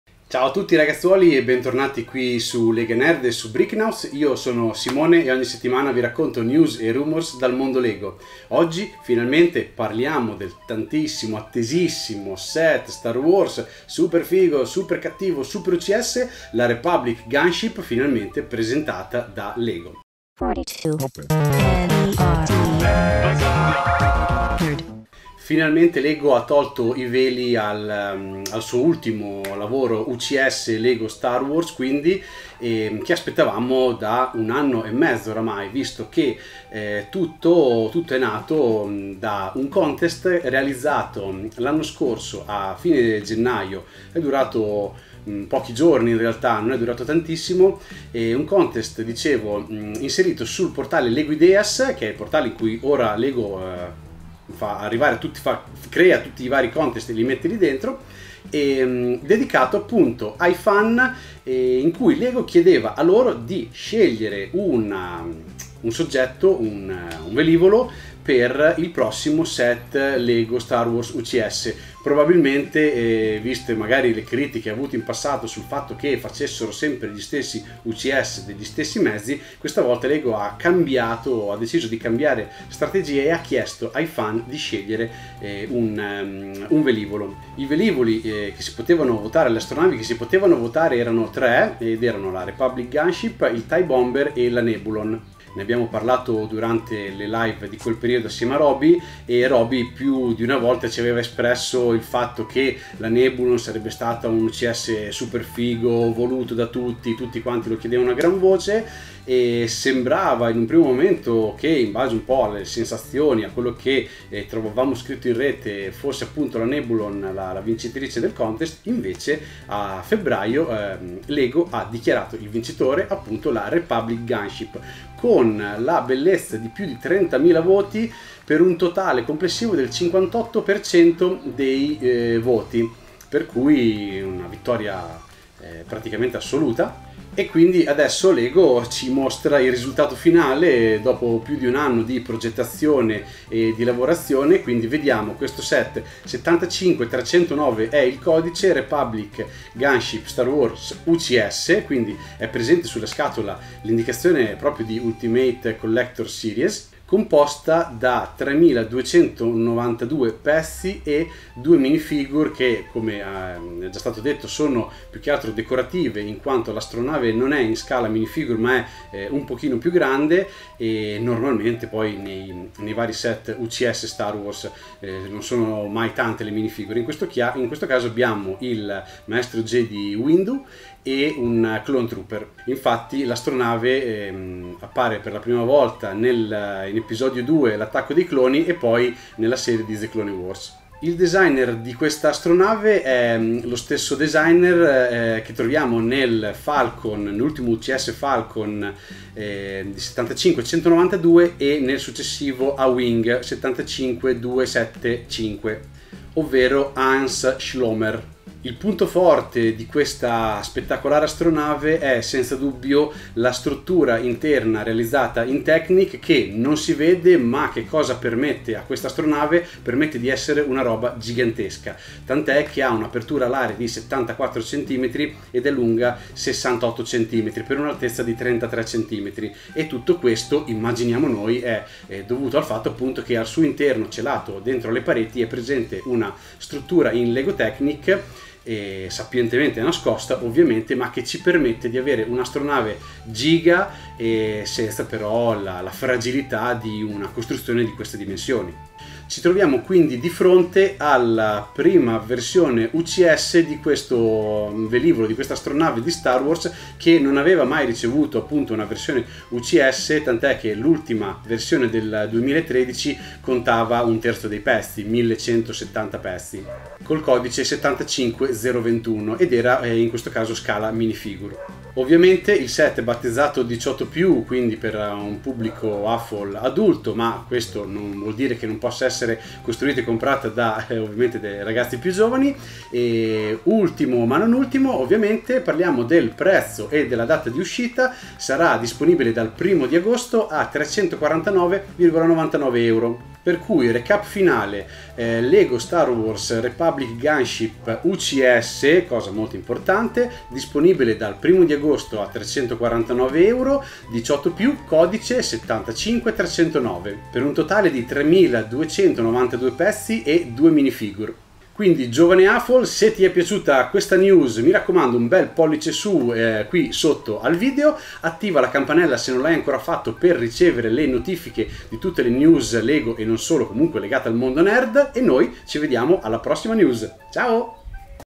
LEGO Star Wars Republic Gunship UCS: il set 75309 scelto dai fan!
LEGO ha annunciato il set Star Wars UCS 75309 Republic Gunship, composto da quasi 3300 pezzi e con un’imponente apertura alare di 72 cm!